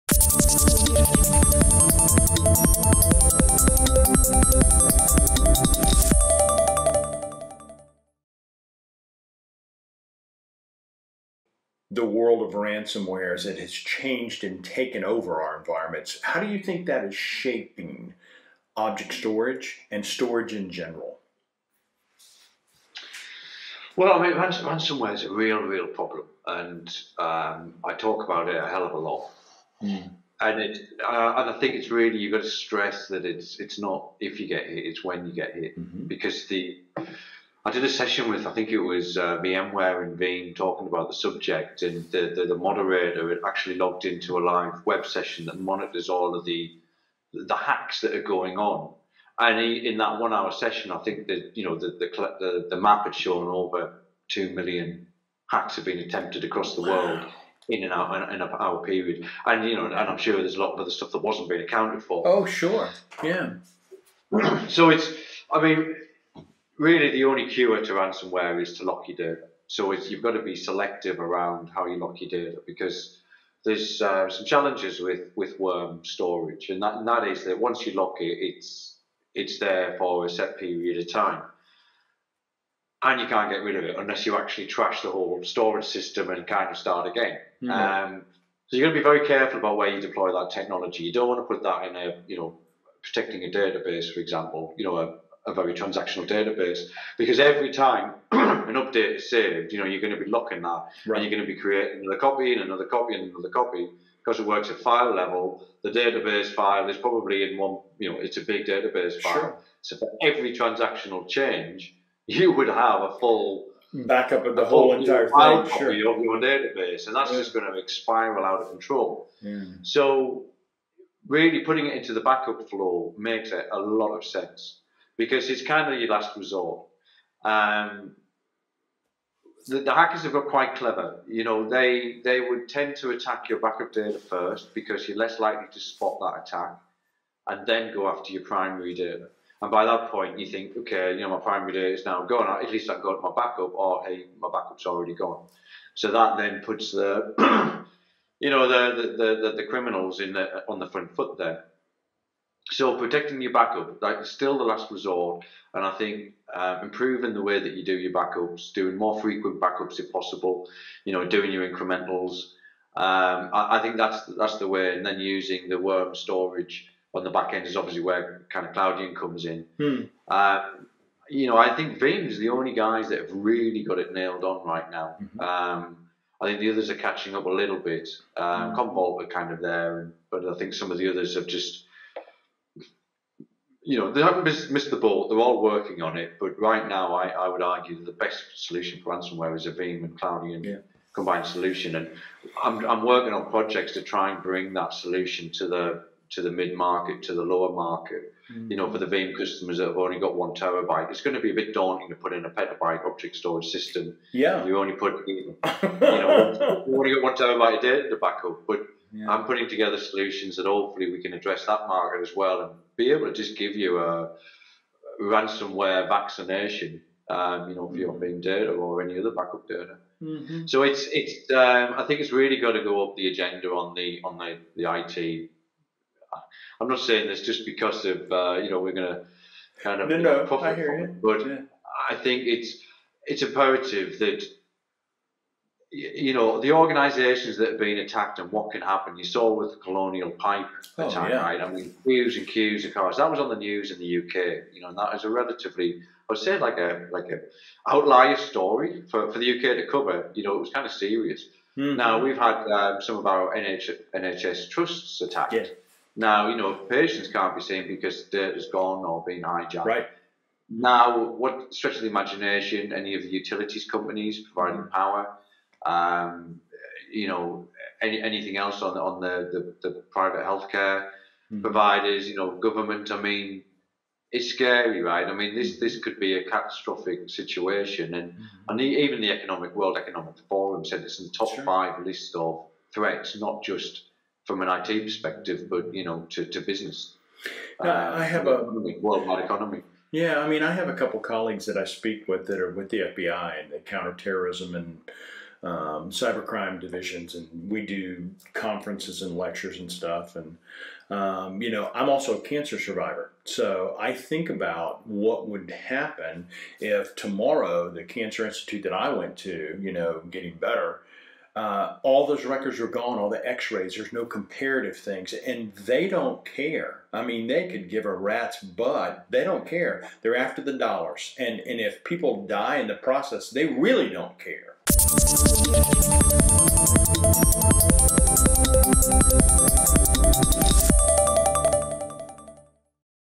Cloudian's Neil Stobart: How to Vaccinate Against Ransomware
How to Vaccinate Against Ransomware
Van Flowers Interviews Neil Stobart, Cloudian VP of Global System Engineering

Looking for an electronica DJ who can vaccinate your company against ransomware? Neil Stobart is your man. Van and Neil go deep on music, family, favorite new tech, the data storage (r)evolution, and of course, what it takes to defeat ransomware. Don't miss this fun, eclectic interview with Neil Stobart!